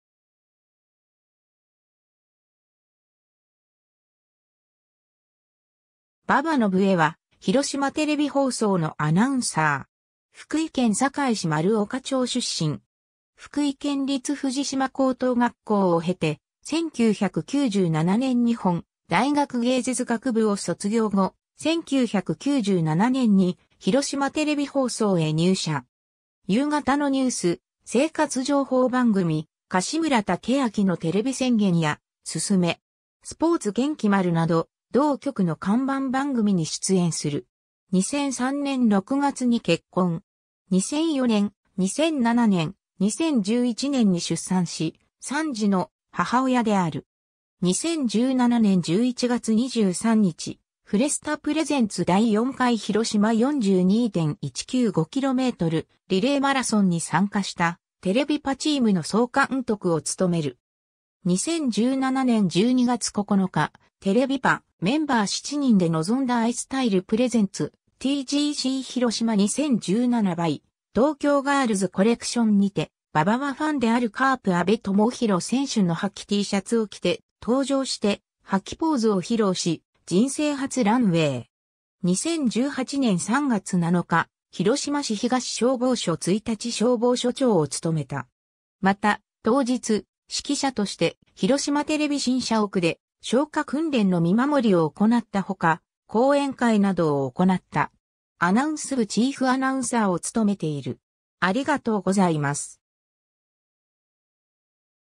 馬場のぶえ
馬場 のぶえ（ばば のぶえ、1975年2月27日 - ）は、広島テレビ放送（HTV）のアナウンサー。
福井県坂井市丸岡町（旧坂井郡丸岡町）出身。
 福井県立藤島高等学校を経て、1997年日本大学芸術学部を卒業後、1997年に広島テレビ放送へ入社。
夕方のニュース・生活情報番組「柏村武昭のテレビ宣言」（その後「テレビ宣言にゅー」→「テレビ宣言」と改題）や「進め!スポーツ元気丸」など同局の看板番組に出演する。
 2003年6月に結婚。
2004年、2007年、2011年に出産し、3児（2男1女）の母親である。
 2017年11月23日、フレスタPresents 第4回ひろしま42.195kmリレーマラソンに参加した『テレビ派』チームの総監督を務める。
 2017年12月9日、『テレビ派』メンバー7人で臨んだIstyle presents TGC HIROSHIMA 2017 by TOKYO GIRLS COLLECTIONにて、馬場はファンであるカープ安部友裕選手の覇気Tシャツを着て登場して覇気ポーズを披露し人生初ランウェイ。
 2018年3月7日、広島市東消防署 一日消防署長を務めた。
また、当日、指揮者として広島テレビ新社屋で消火訓練の見守りを行ったほか、講演会などを行った。
 アナウンス部チーフアナウンサーを務めている。